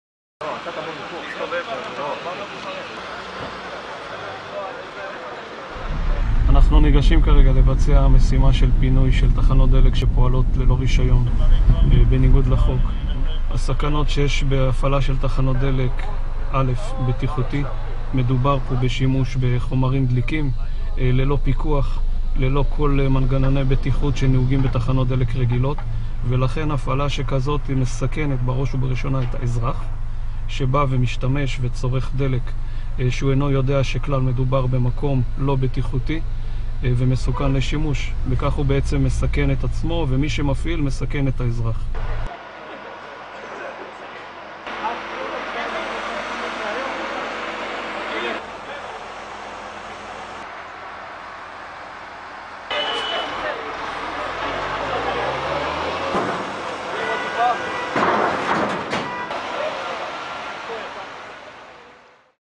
Israel: La police a fermé trois stations d'essence illégales
Démantèlement de trois stations-service dans le village d'Ein Mahal, dans le nord du pays. Les équipements et le carburant ont été confisqués et des actes d'accusation ont été déposés contre les propriétaires.